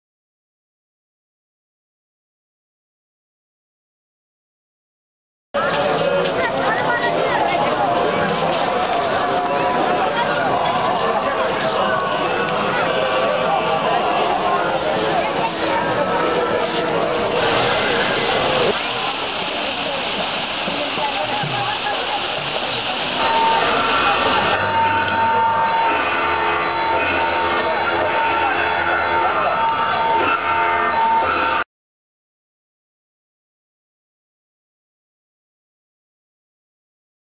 Around Mahabodhi Temple.
Around Mahabodhi Temple, There were so much crowded with Buddhists for their Religious Visit. And many people were doing curtsy in front of the Buddha Staus. I kept pious mind not to disturb Buddhists.